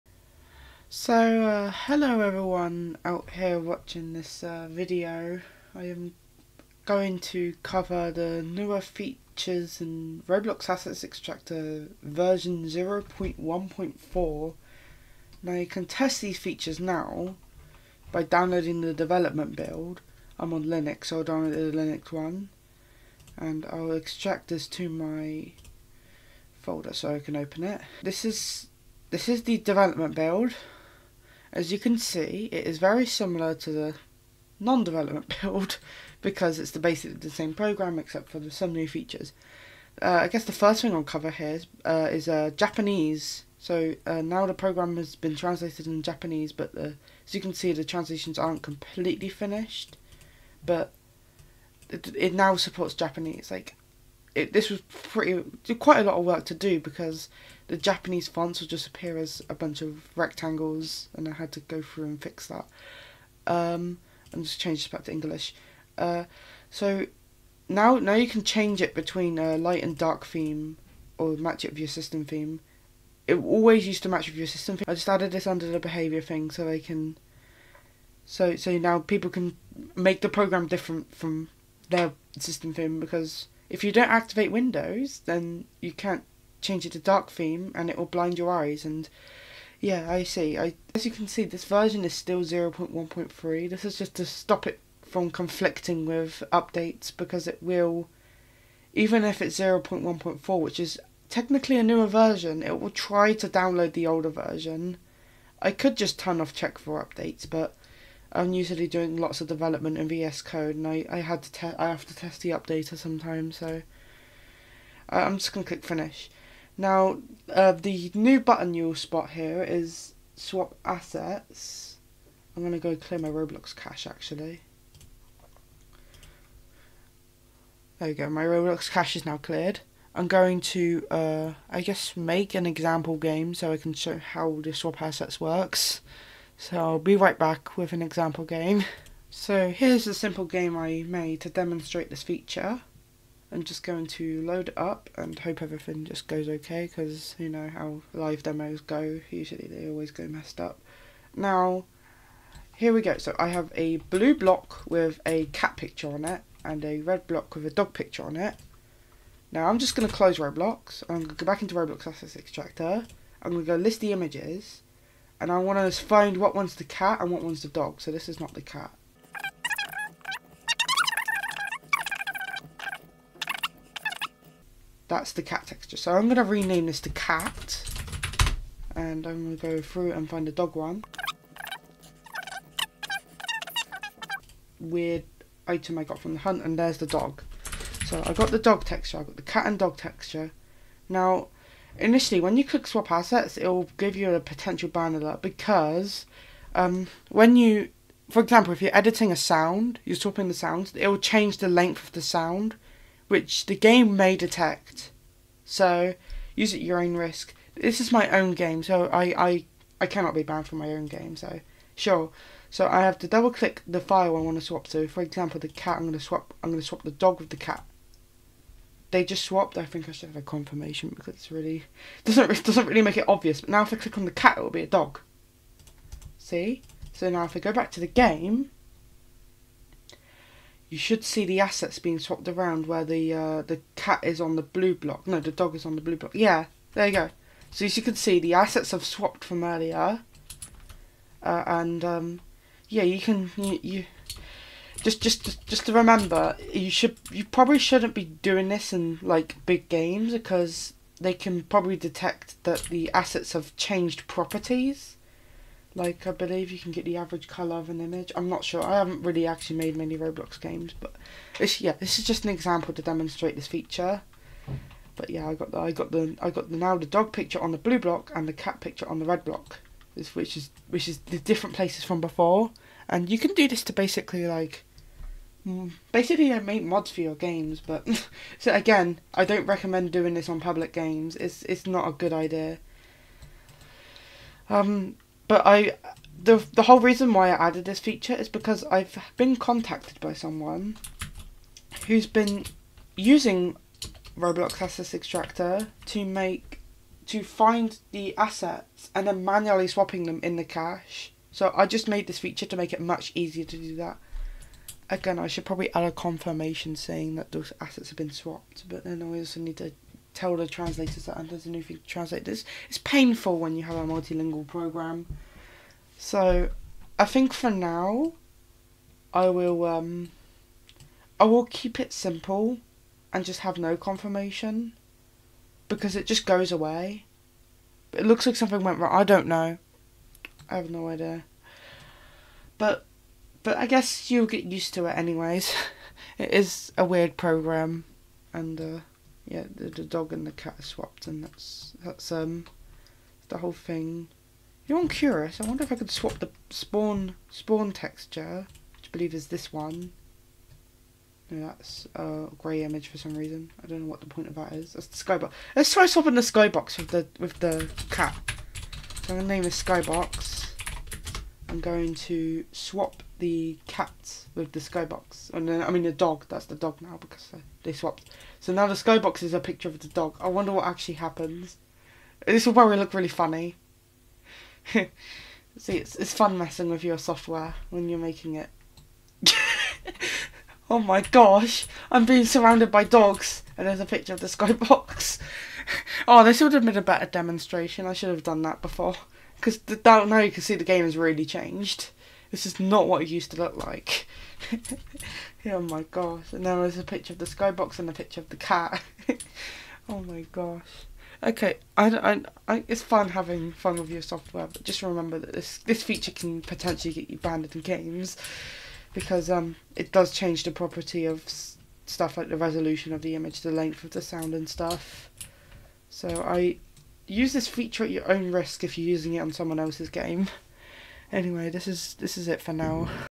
Upcoming features in Roblox Assets Extractor v0.1.4
You can test these features now by downloading the development build